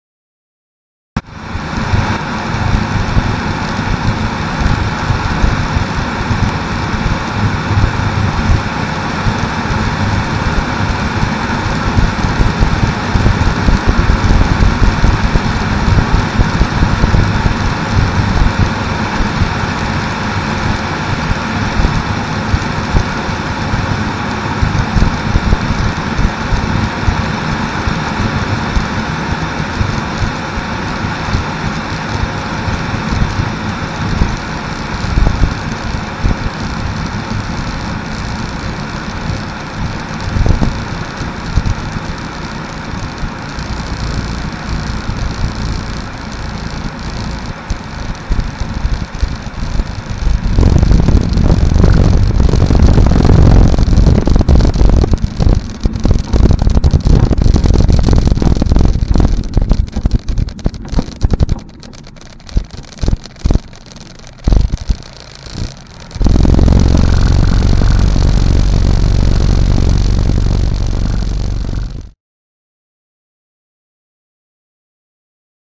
L'ombre d'un atterrissage. The shadow of a landing
Atterrissage avec des "S" de perte d'altitude à Albertville (Savoie) piste 05. L'ombre et le  Cordouan HM 1100 se rejoignent sur la piste.